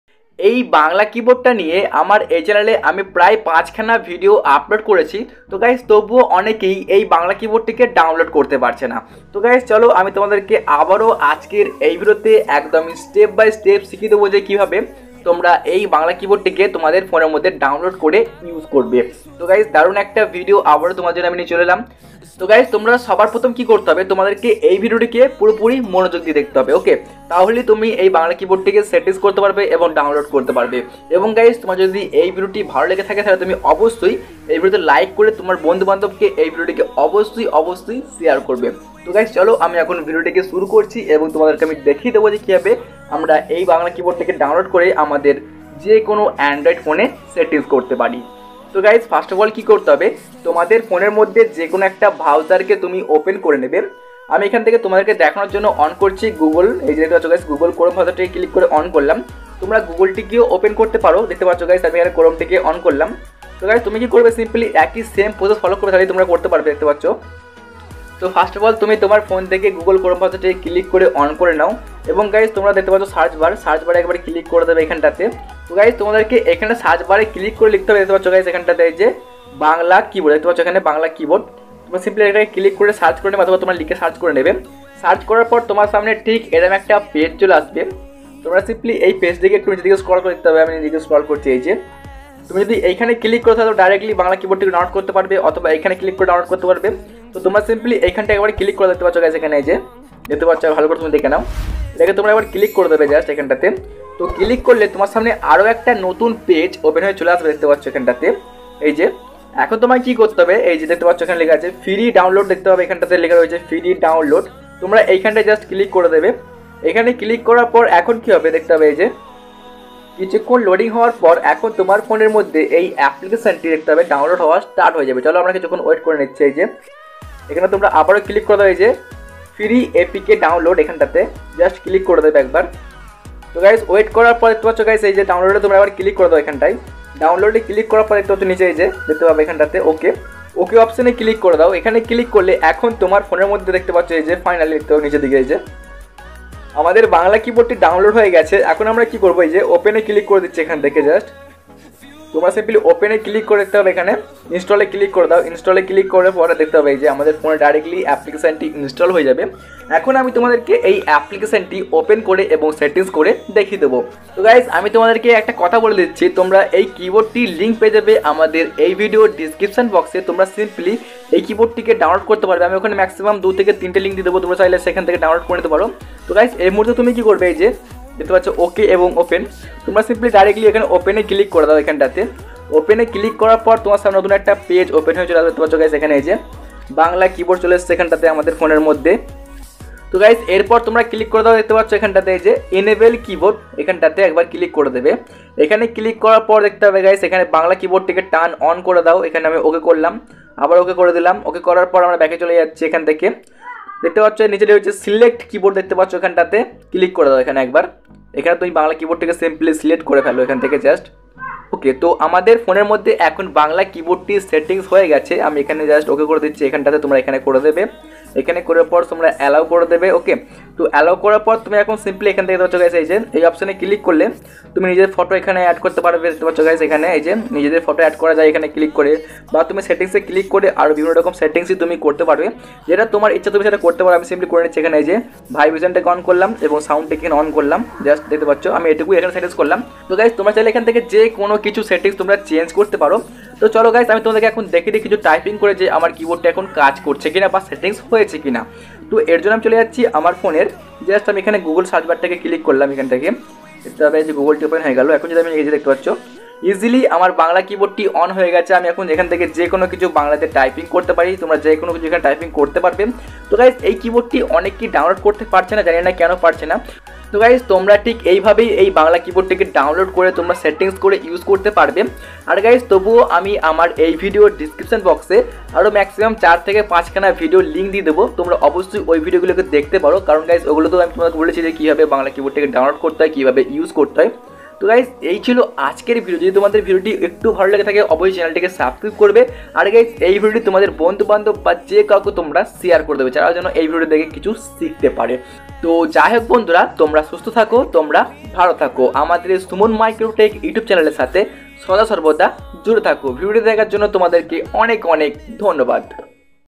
Bangla Keyboard Download | Bangla Keyboard Kivabe Set Korbo
ai video te tomader bolechi je kivabe bangla keyboard download korte hoy akdom stap by stap